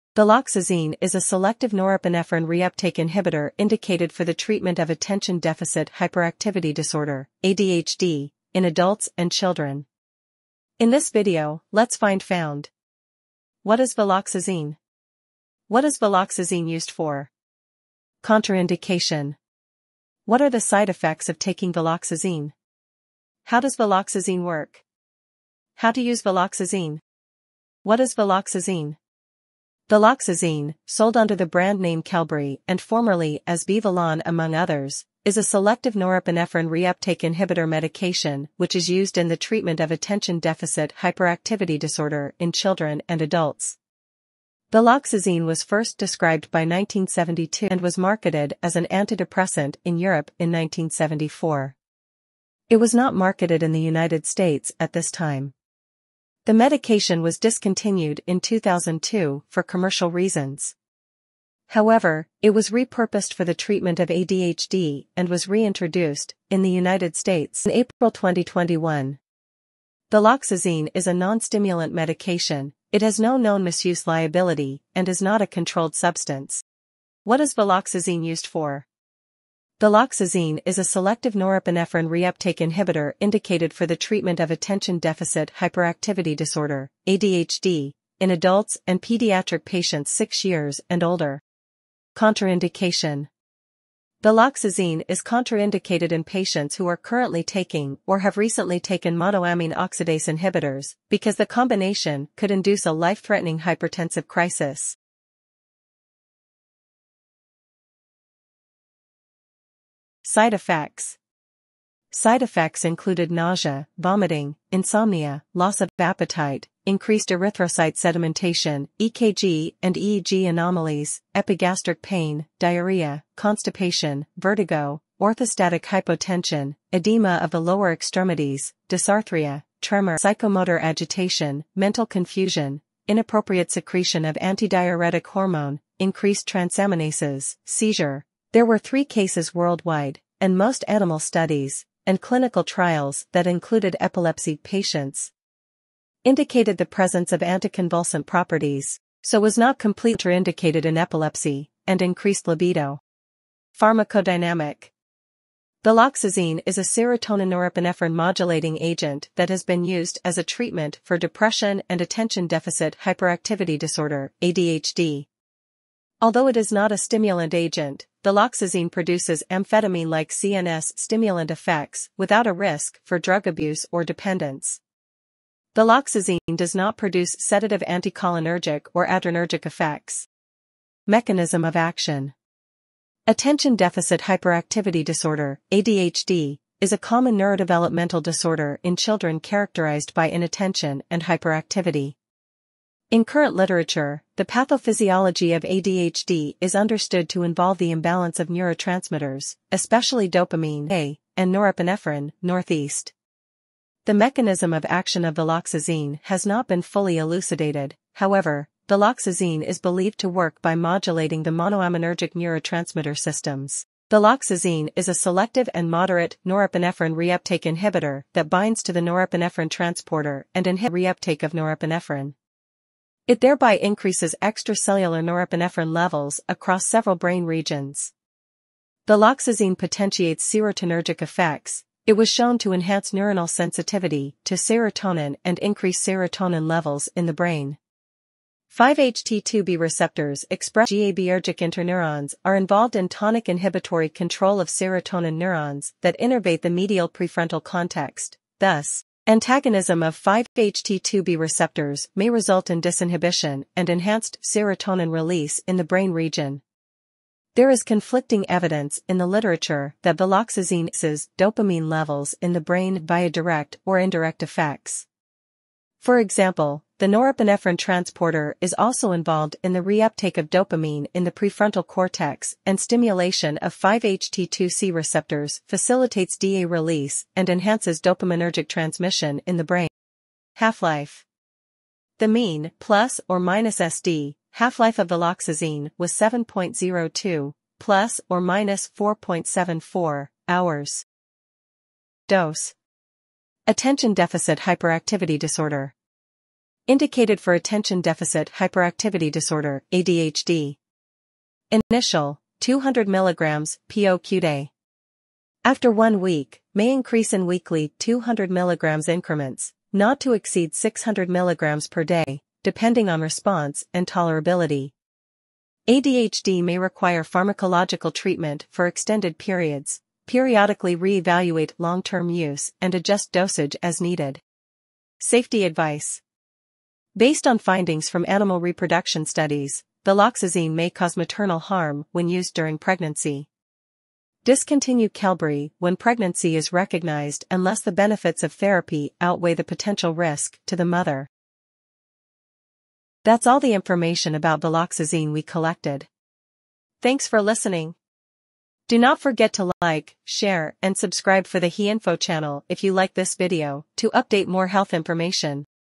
Viloxazine | Uses, Dosage, Side Effects & Mechanism | Qelbree
Viloxazine is a selective norepinephrine reuptake inhibitor indicated for the treatment of Attention Deficit Hyperactivity Disorder (ADHD) in adults and children.

In this video, let’s find found:
What is Viloxazine?
What is Viloxazine used for?
Contraindication
What are the side effects of taking Viloxazine?
How does Viloxazine work?
How to use Viloxazine?

📚 Sources / References
•  Medication information (uses, pharmacodynamics, mechanism of action, half-life):